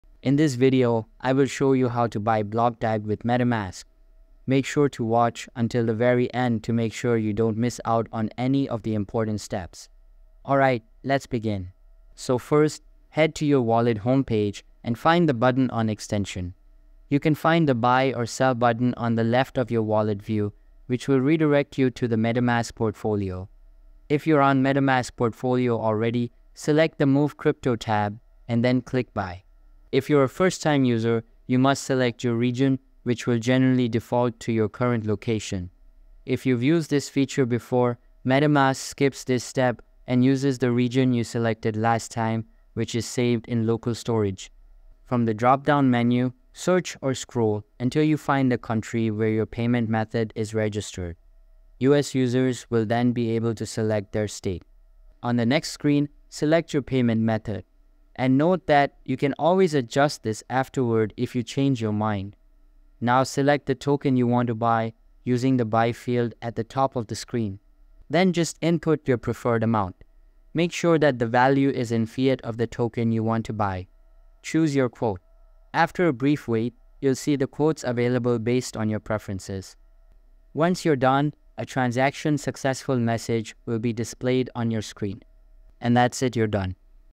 NEW!HOW TO EASILY BUY BLOCKDAG WITH METAMASK 2025!(FULL GUIDE)
Looking to buy BlockDAG using MetaMask in 2025? This full guide will show you the easiest way to purchase BlockDAG securely through MetaMask. Learn how to connect MetaMask to a decentralized exchange (DEX), find BlockDAG, and make the purchase in just a few simple steps. Whether you're new to MetaMask or a seasoned crypto trader, this guide will help you buy BlockDAG quickly and safely.

Follow these instructions to ensure your BlockDAG transaction is smooth and error-free!

👉 Make sure to watch the whole video to not miss out on any of the important steps!